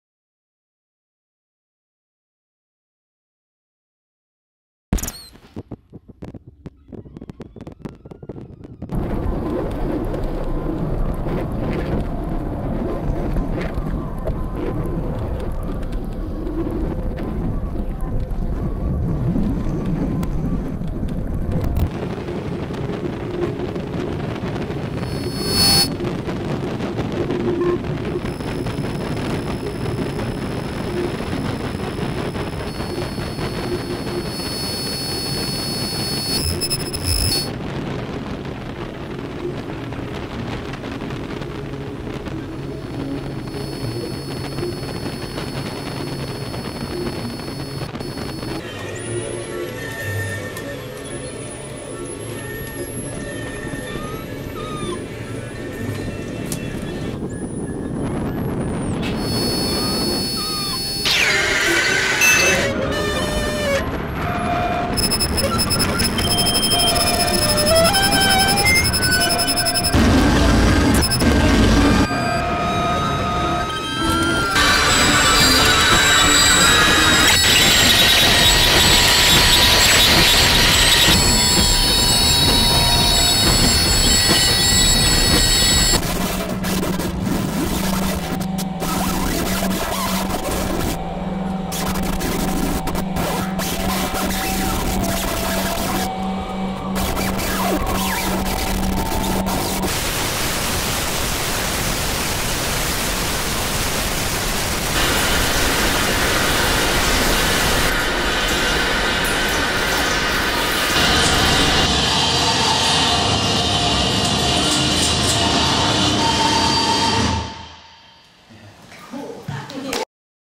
/// BOLD /// Extrait Performance sonore 2 /// Espace im Média 2011
Extrait de la performance sonore du trio BOLD (A. Bellavance + N. Bernier + É. D'Orion)
dans le cadre d'Espace im Media 2011